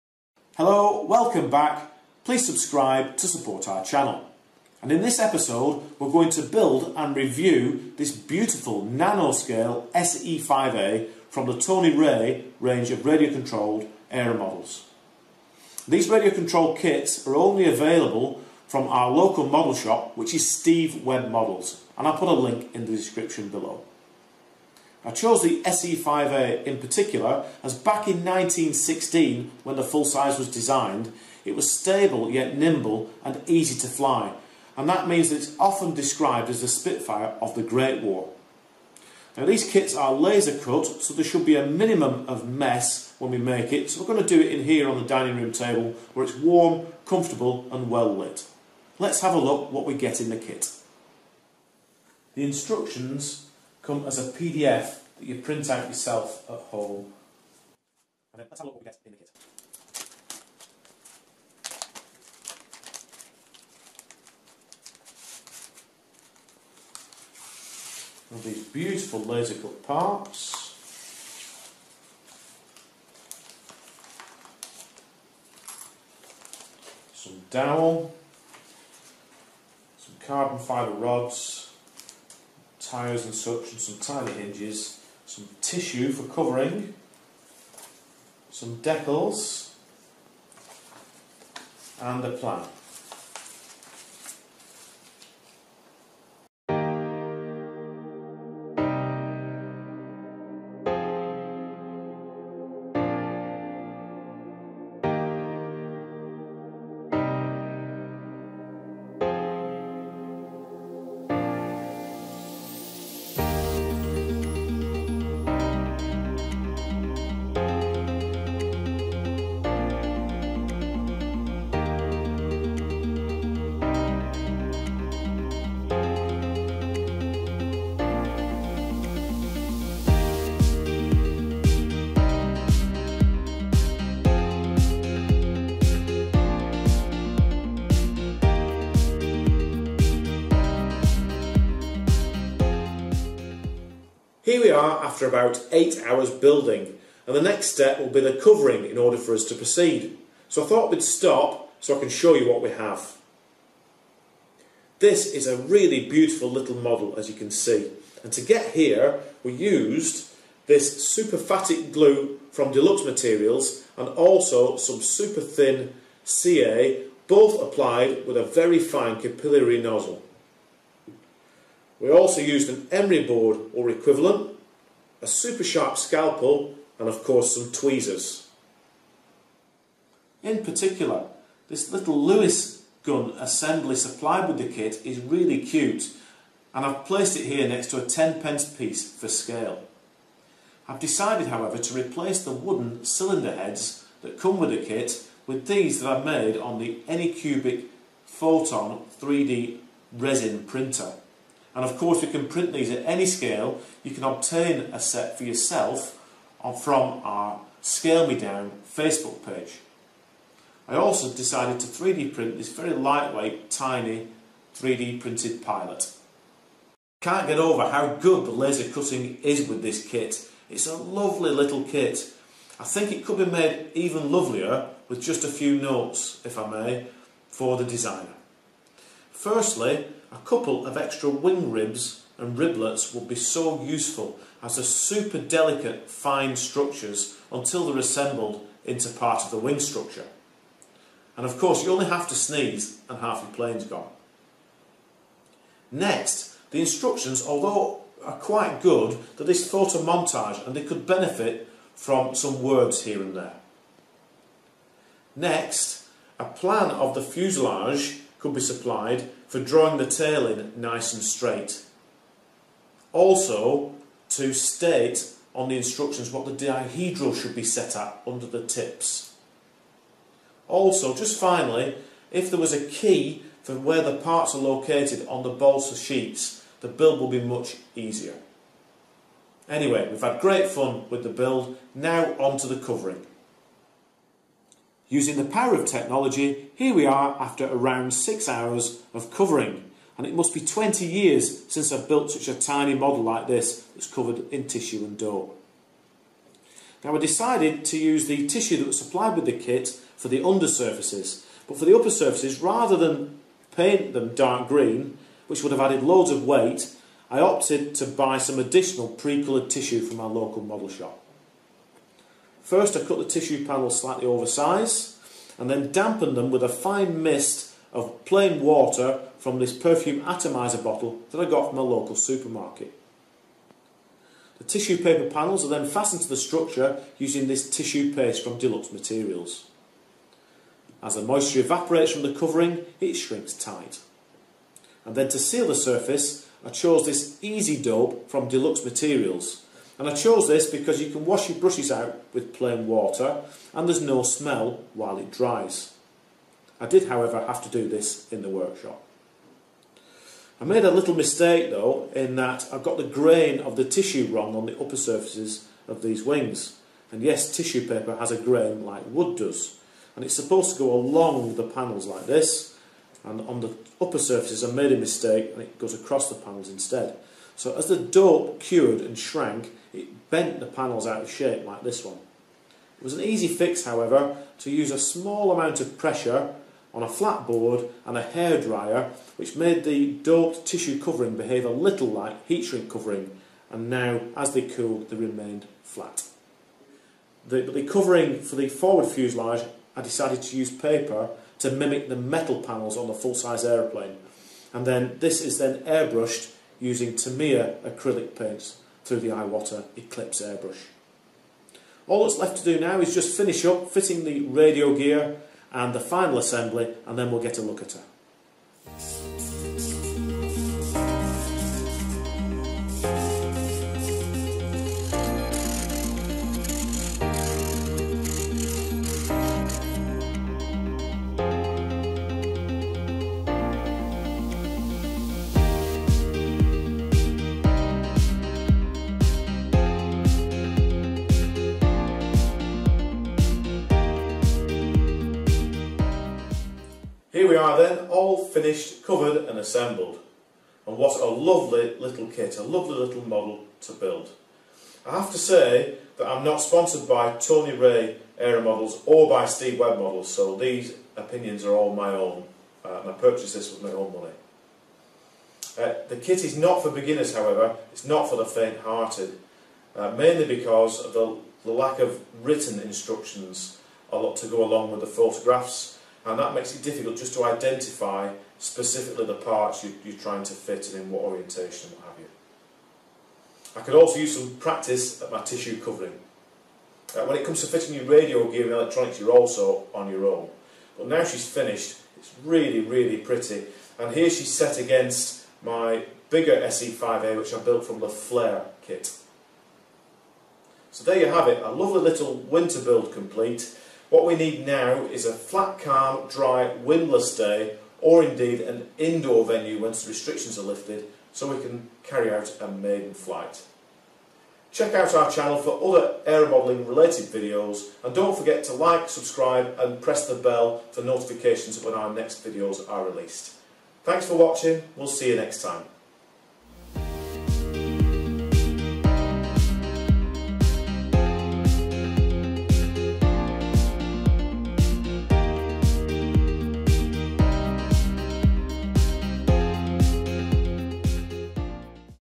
Micro RC Tony Ray SE5a Build and Review
Building the SE5a from the Tony Ray range of peanut scale RC models.  This is a tiny 3 or 4 channel RC model for calm outdoor, or indoor flying.

Links;
3D printed parts available;